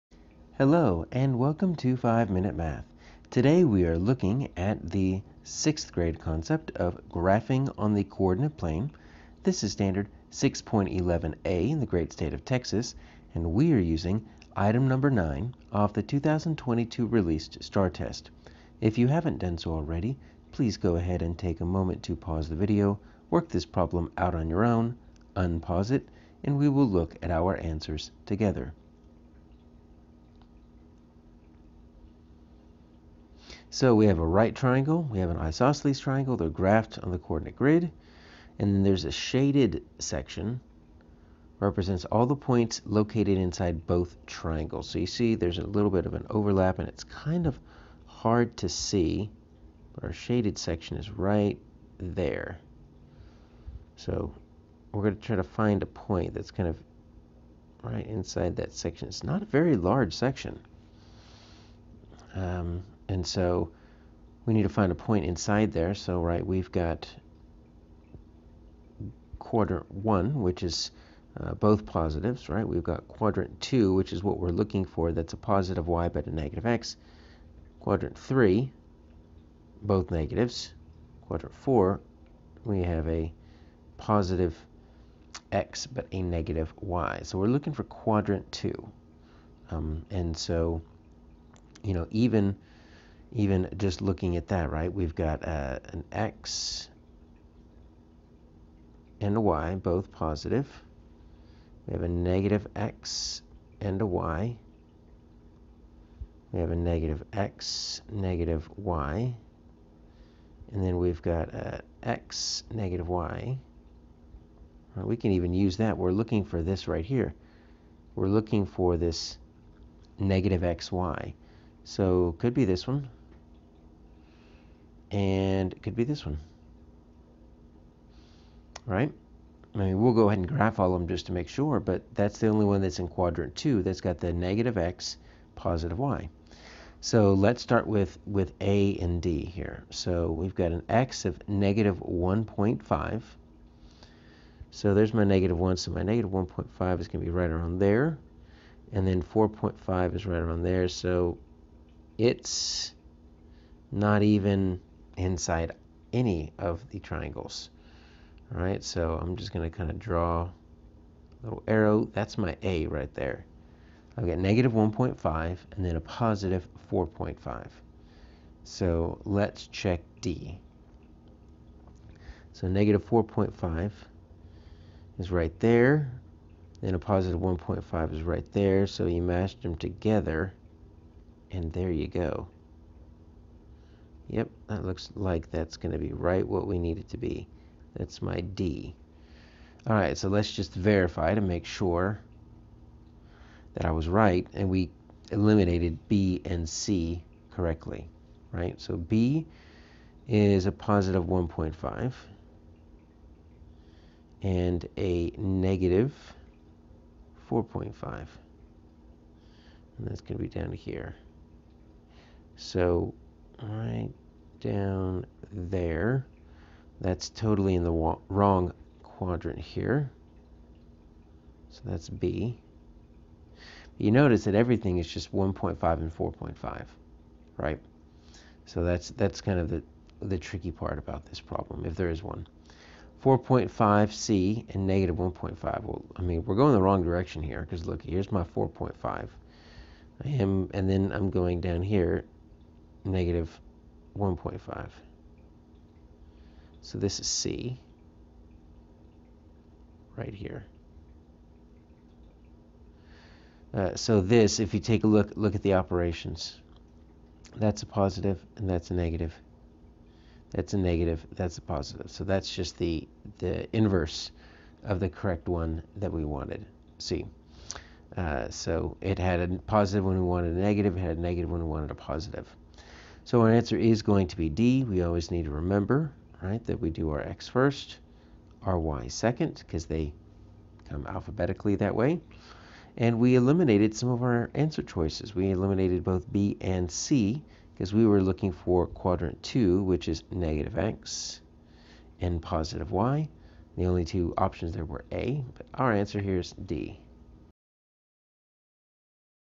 6th Grade STAAR Practice Graphing on the Coordinate Plane (6.11A - #7)
Practice graphing points in all four quadrants using ordered pairs of rational numbers with a test item from the 2022 released STAAR test (#9 - 6.11A).